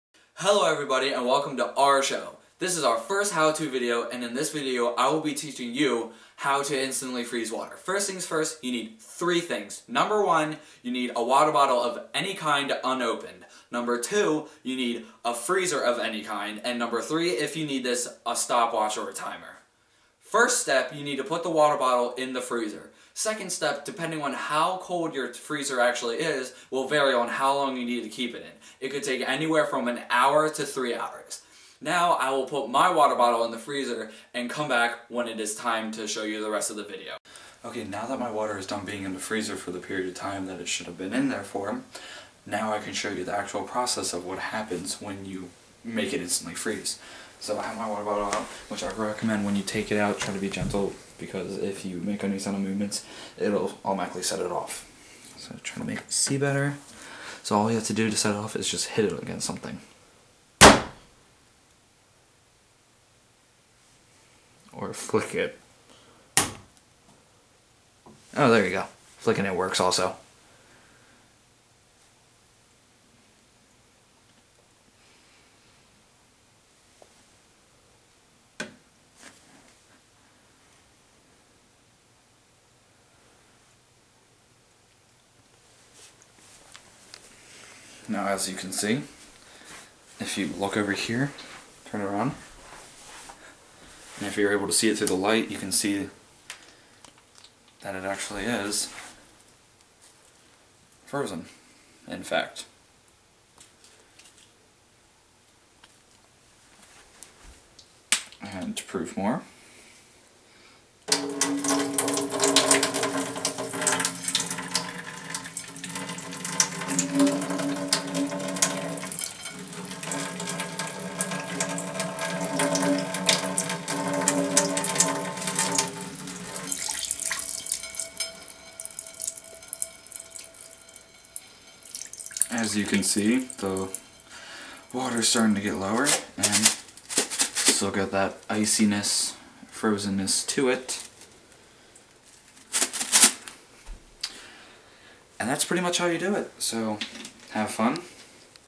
How to Istantly Freeze Water
In this video Andrew explains the process for super cooling a water bottle and making the water inside freeze right before your eyes!